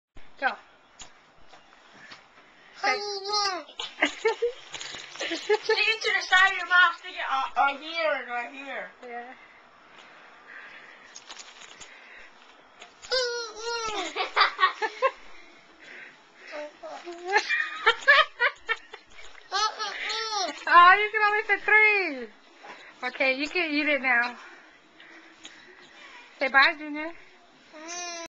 Junior Chubby Bunny
Aww cutie Junior got 3 chubby bunnies